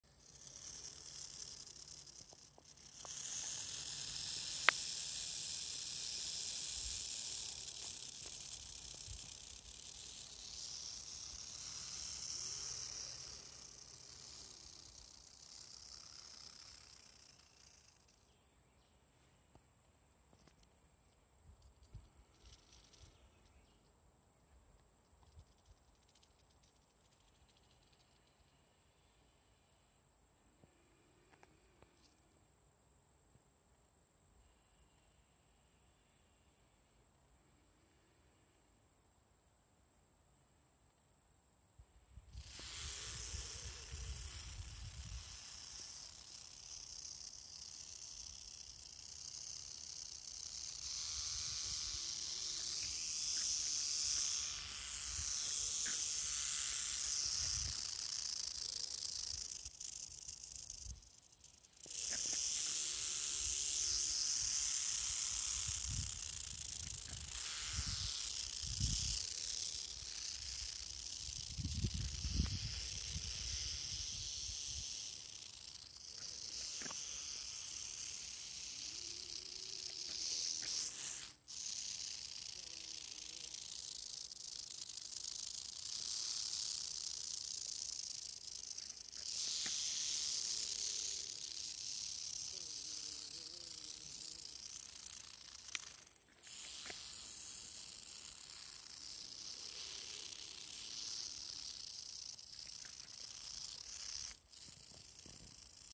Huge Diamondback rattlesnake crossing the road.
Stopped to make sure this little guy made it safely across the road.